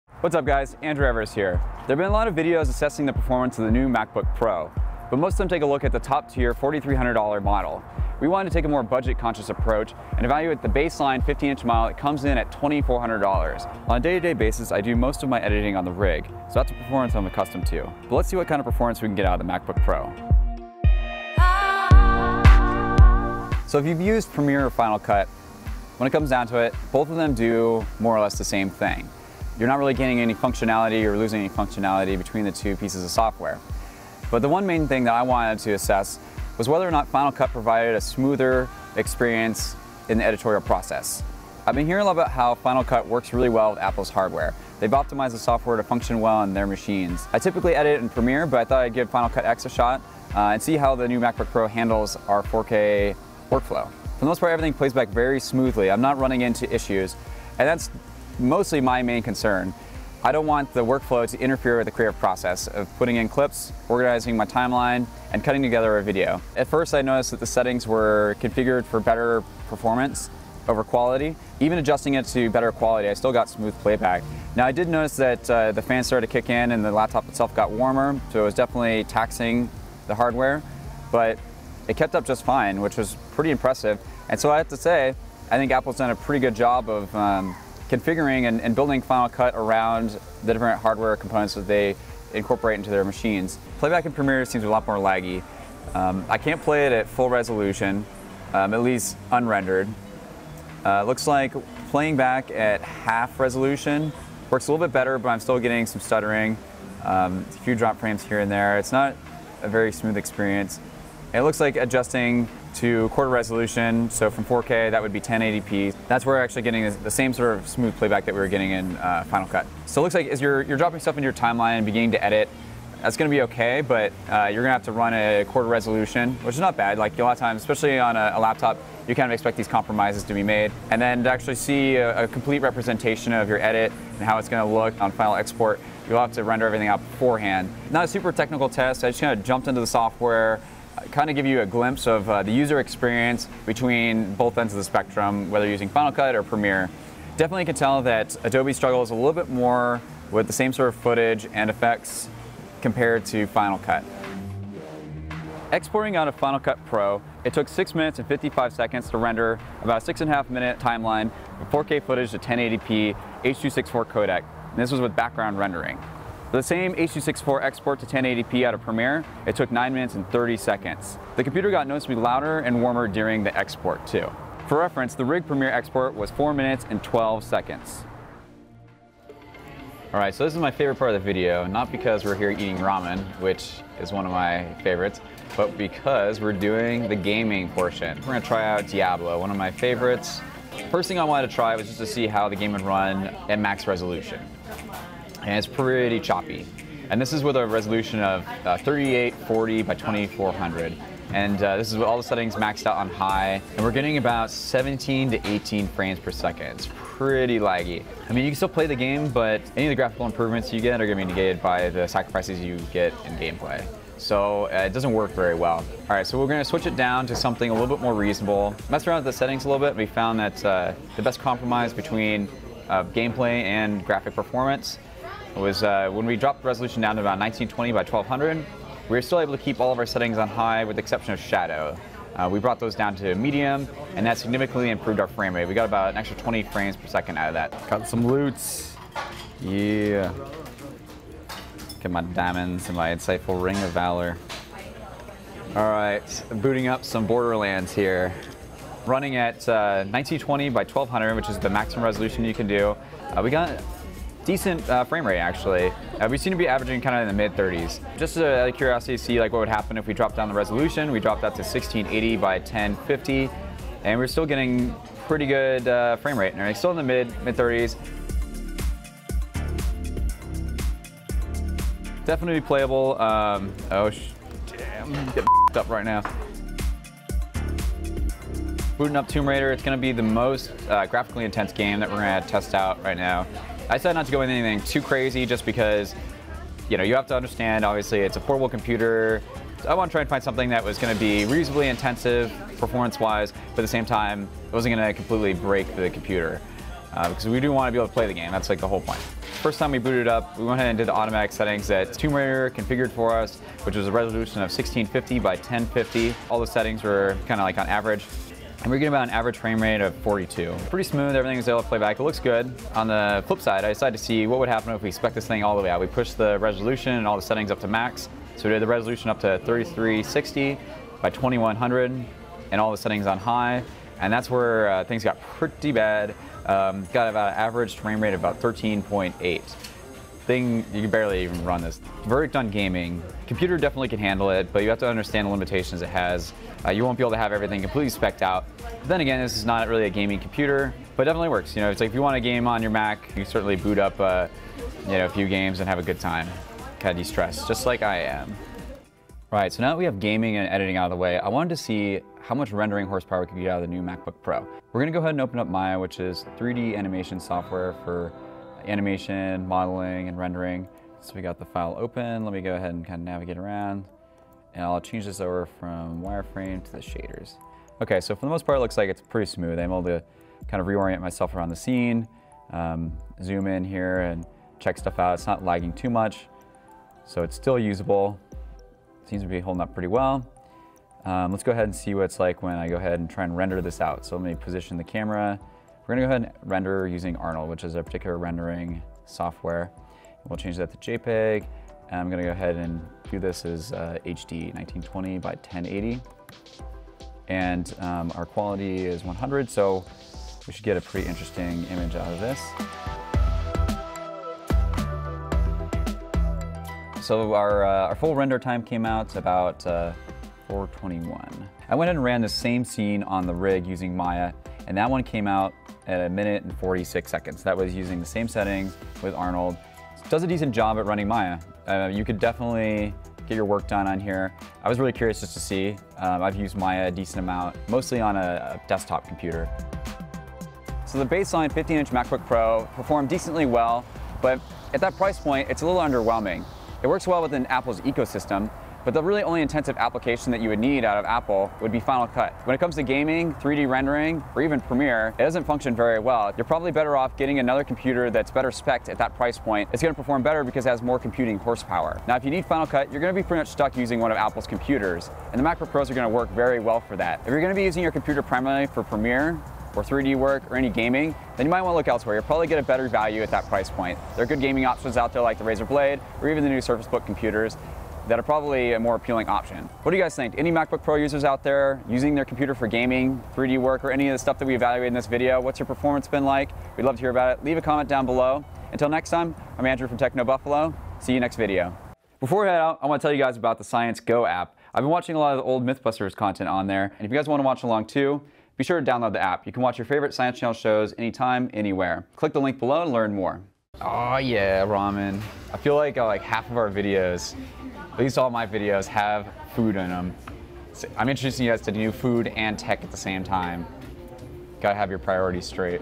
What Can the MacBook Pro 15" Handle?
Is the new MacBook Pro with Touch Bar really a pro machine?